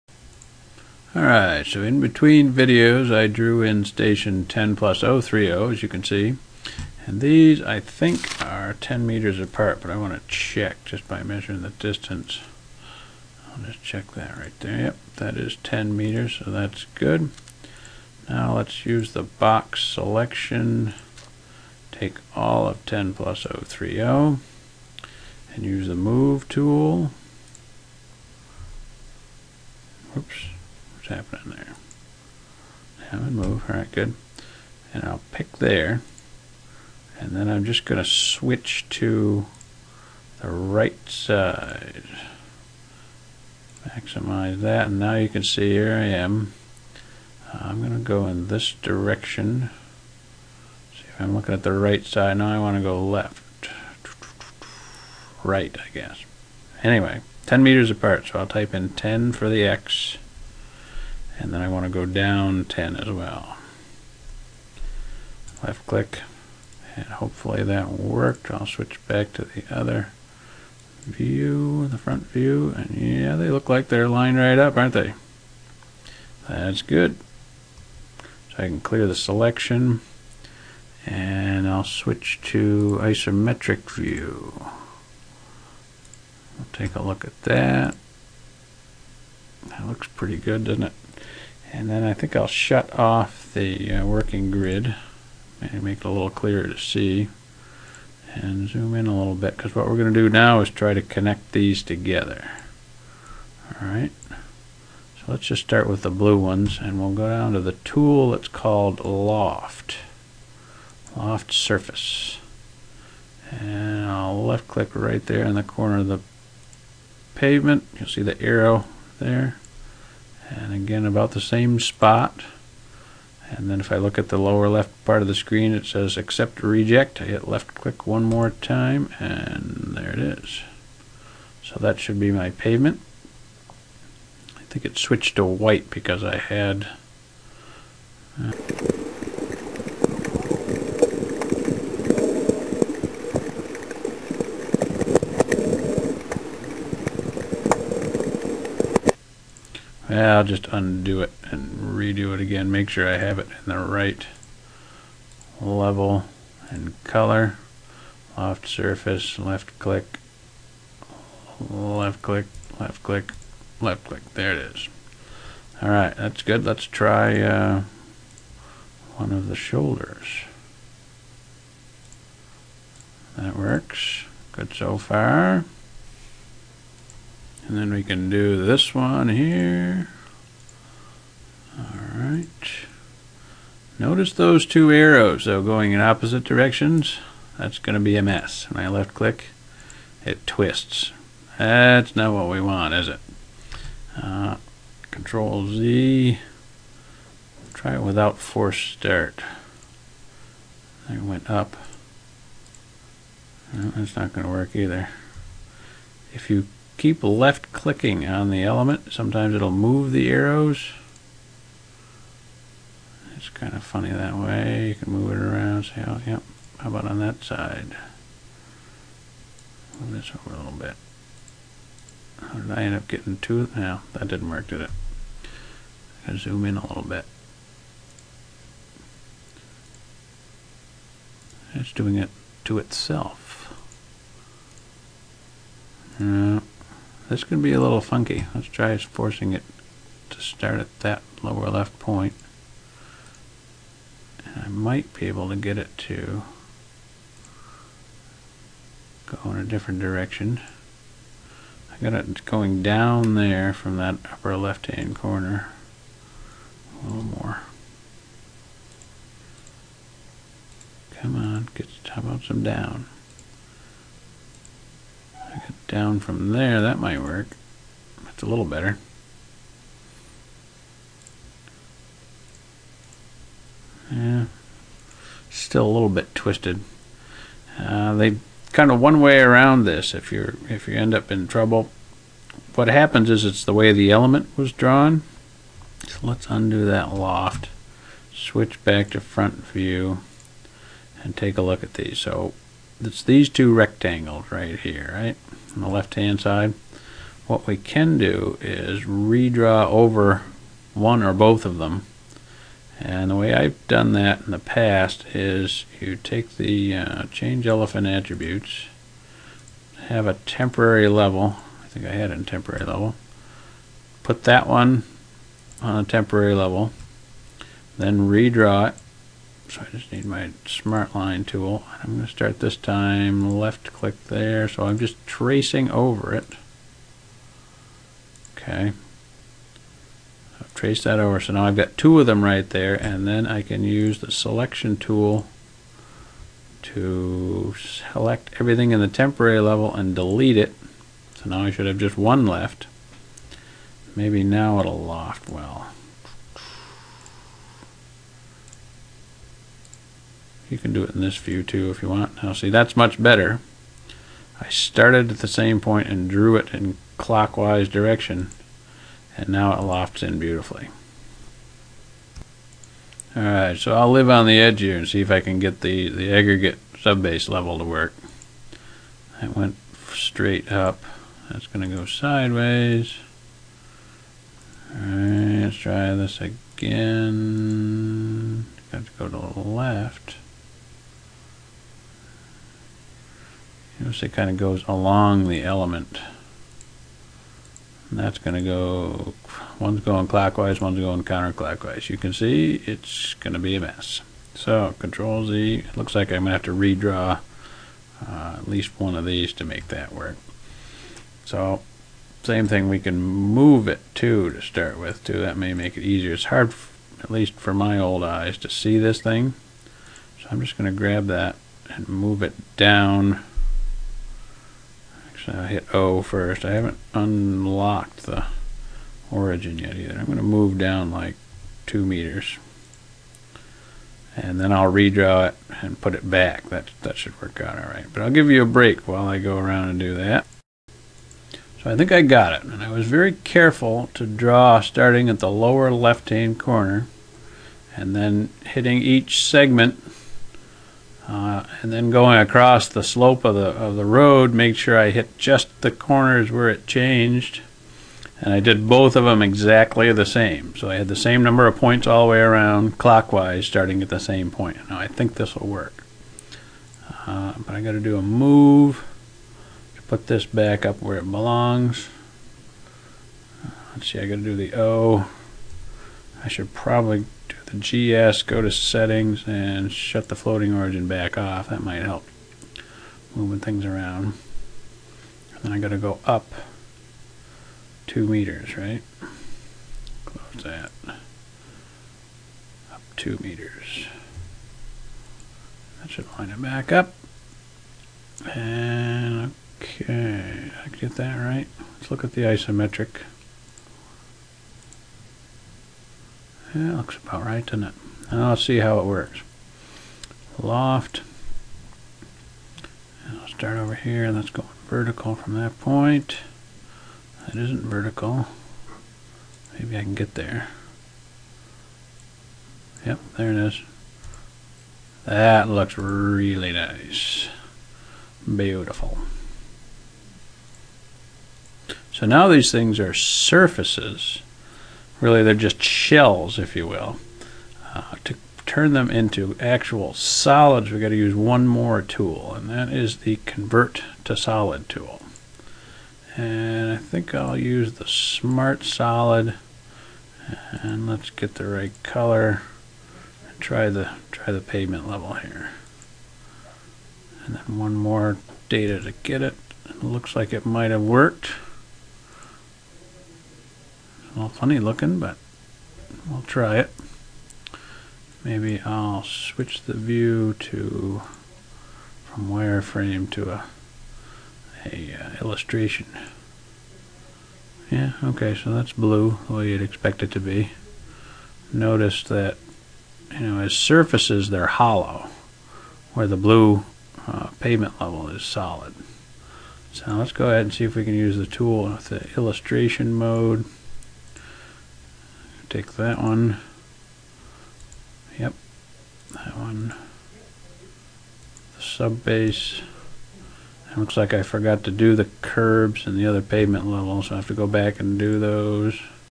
Microstation lecture 14 (CIE 101, Civil Engineering Graphics)
3D road section part 2 of 2.  Connect the two profiles with the Loft tool, tips and tricks to make it work, converting from surfaces to solids.